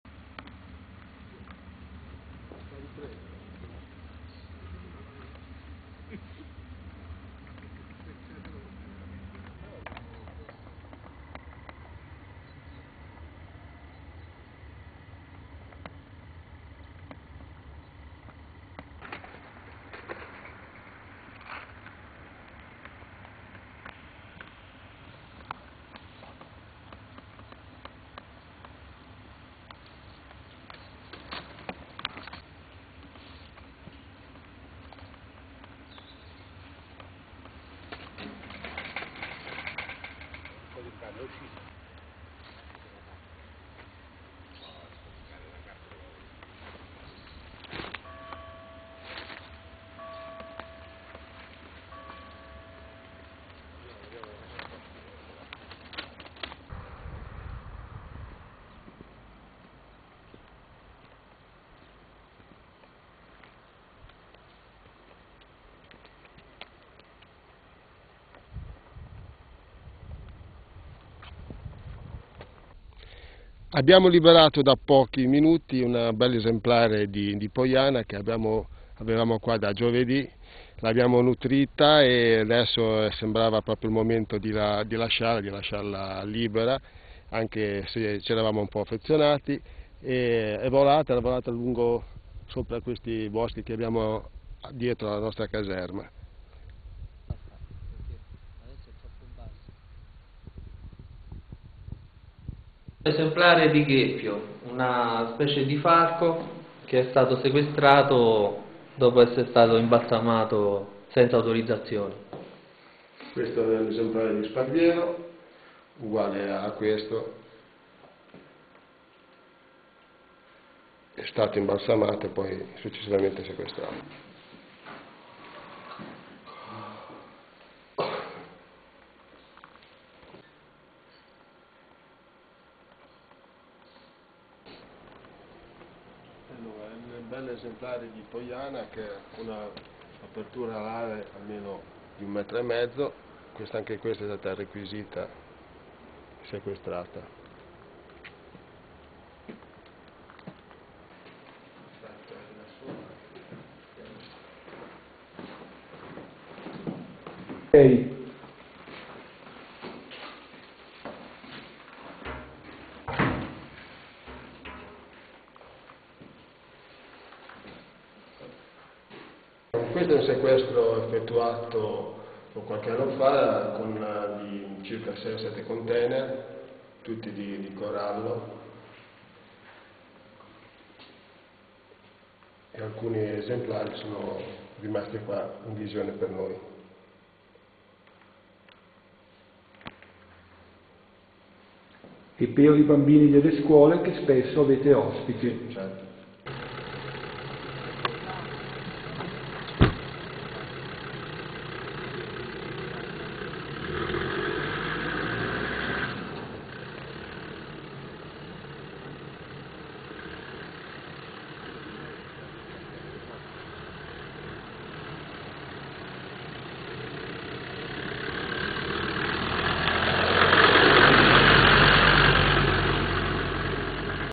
LIBERATO DAL CORPO FORESTALE IL RAPACE ORMAI GUARITO
Video of Vittorio Innocente - After a few days, the Buzzard looked after by Forestry at the barracks of Borghetto Vara, today chose freedom .. In order not to disturb it, the roof of the large cage was a hole through which the young specimen came out, after a few moments of hesitation he flew towards the thick bush. I am saddened agents of CFS, but happy to have recovered this bird of prey. The barracks Borghetto Vara is one of the few to be equipped helicopter. Often hosts school groups to which the activity is explained by the Forestry and shown the interesting material seized.

Video di Vittorio Innocente - Dopo pochi giorni, la Poiana accudita dai Forestali presso la caserma di Borghetto Vara, oggi ha scelto la libertà.. Per non disturbarla, nel tetto della grande gabbia è stato praticato un foro attraverso il quale il giovane esemplare è uscito, dopo pochi attimi di esitazione è volato verso la  fitta boscaglia. Sono rattristati gli agenti del C.F.S., ma felici di aver recuperato questo rapace. La caserma di Borghetto Vara è una delle poche ad essere dotata dell'elicottero. Spesso ospita scolaresche alle quali viene spiegata l'attività svolta dai Forestali e mostrato l'interessante materiale sequestrato.